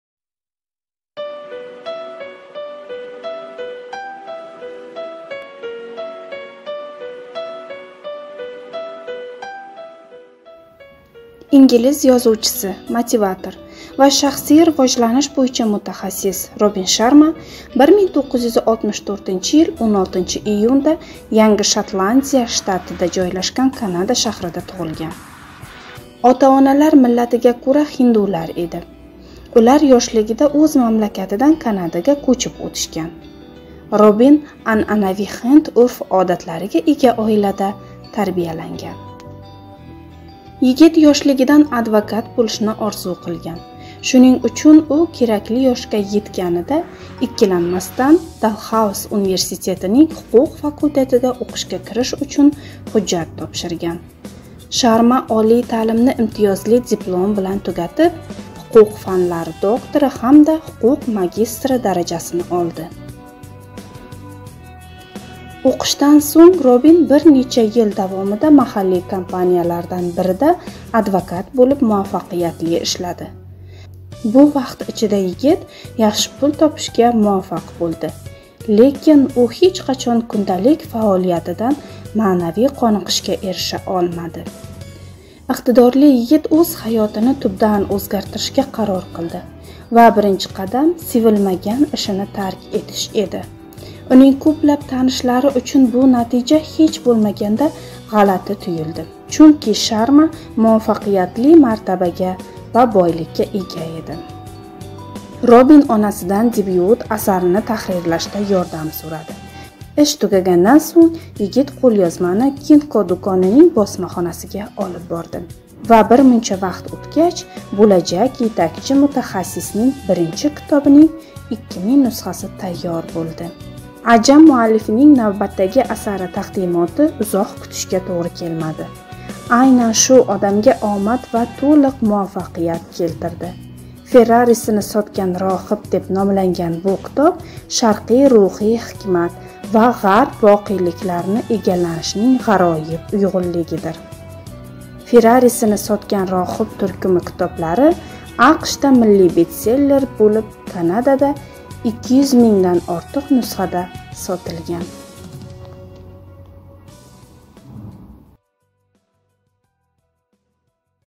Ingliz yozuvchisi, shaxsiy rivojlanish boʻyicha mutaxassis Robin Sharmaning hayot va ijod yo'li
Siz ushbu kanal orqali bizning foaliyatimiz haqida ma'lumotlarga va yangiliklarga ega bo'lishingiz mumkin